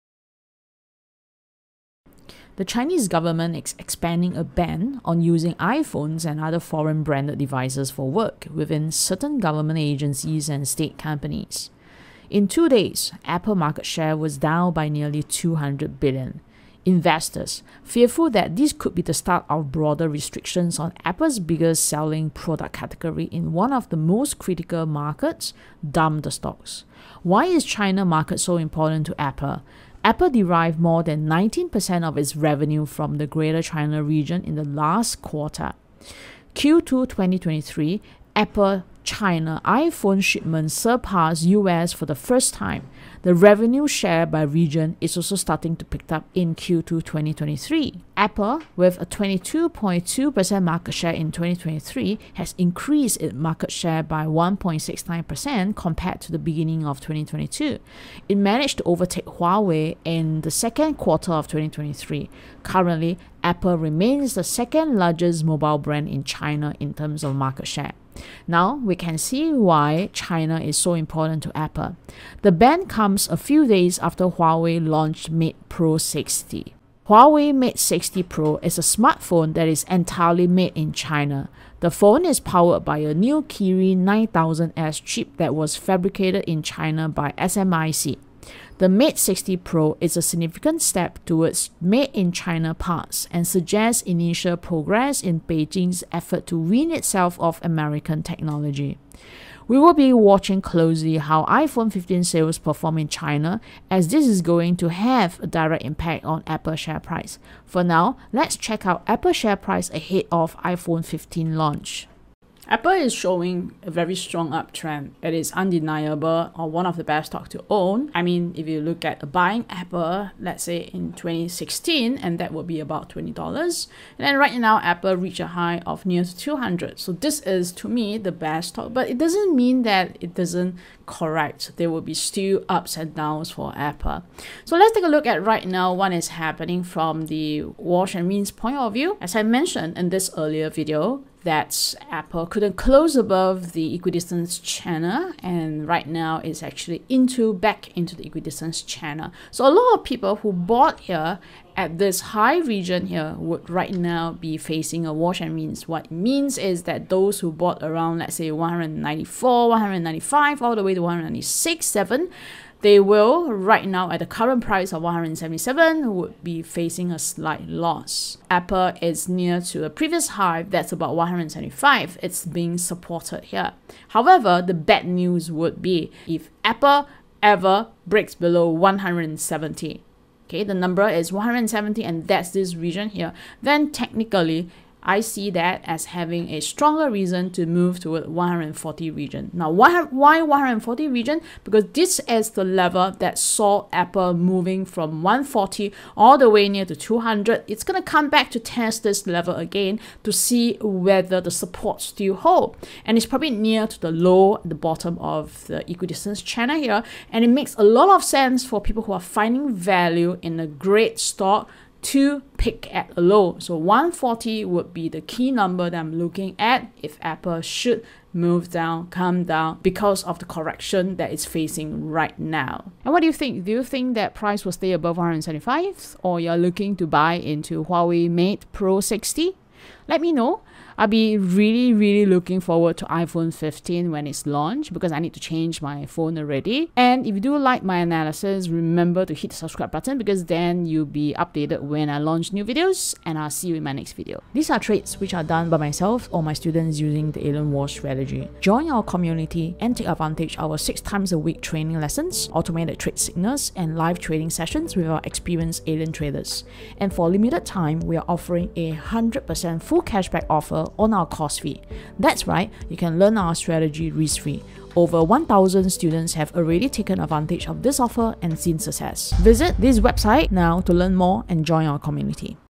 What Iphone 15's Ban In China Could Mean For Apple AAPL Stock?
If China Bans iPhone 15, this could be bad for Apple Stocks? I'm not sure, because government and state companies might be a small number for the total sales of iphones. In this video I have included 2 key levels to watch out for APPL share. One of the levels is very near and is something to watch ahead of iphone 15 launch.

The Chinese government's ban on iPhones for government officials could hurt Apple's iPhone 15 stocks. The ban could dampen demand for the iPhone 15 in China, which is Apple's second-largest market. Investors should closely monitor the situation.

The ban is a major blow to Apple, which has long relied on China as a major market for its products. Apple sold over 40 million iPhones in China last year, making it the company's second-largest market after the United States.

00:00 Introduction
00:28 Why is China market important to Apple?
01:17 Huawei Mate 60 Pro
01:52 Apple Inc Chart
03:43 Ending remarks